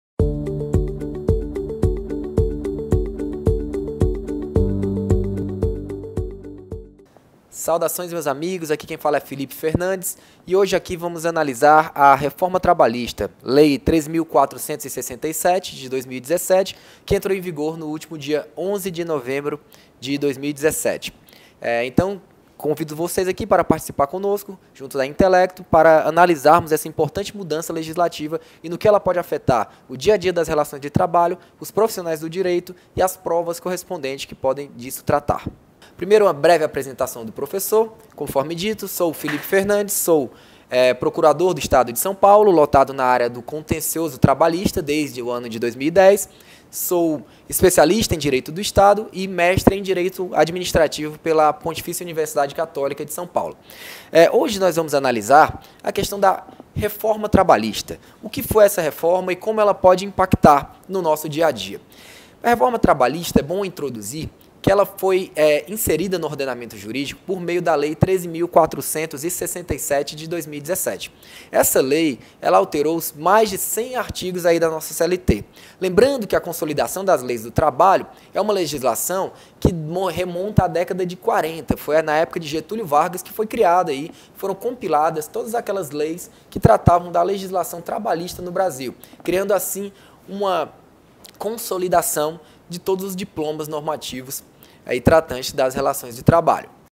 Introdução à Reforma Trabalhista
INSCREVA-SE no Canal Com a nova Reforma Trabalhista, o operador do direito deve se atualizar, partindo deste pressuposto, a Intelecto Educação oferece um novo curso completo, sobre a tal reforma, não perca tempo!